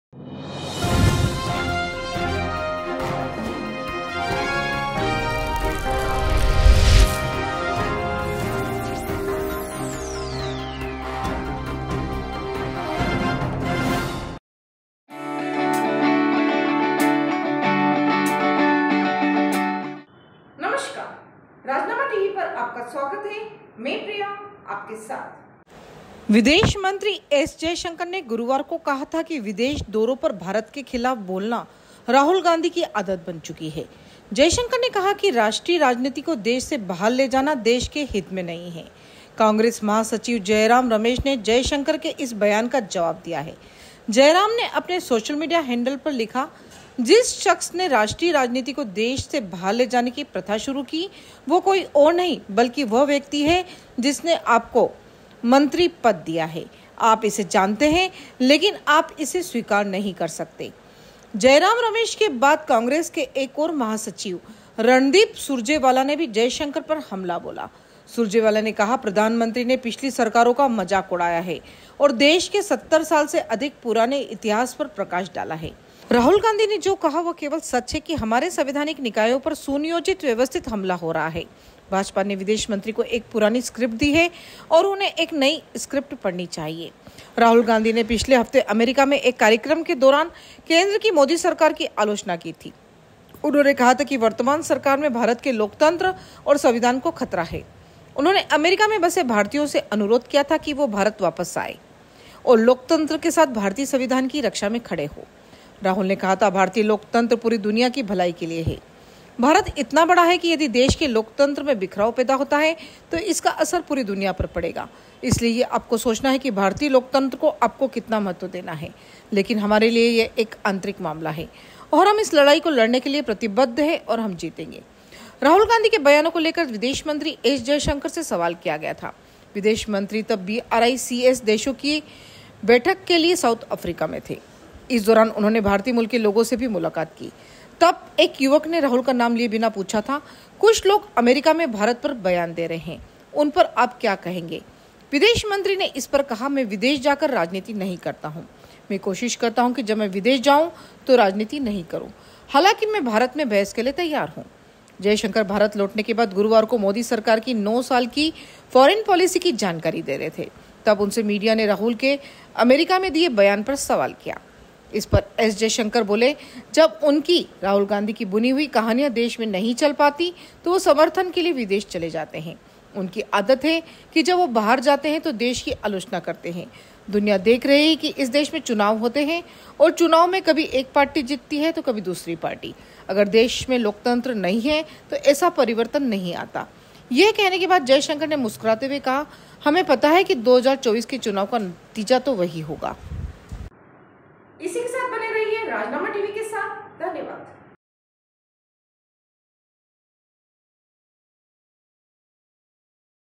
anchorpriya राहुल पर जयशंकर के बयान का जयराम ने दिया जवाब
About this video:राहुल पर जयशंकर के बयान का जयराम ने दिया जवाब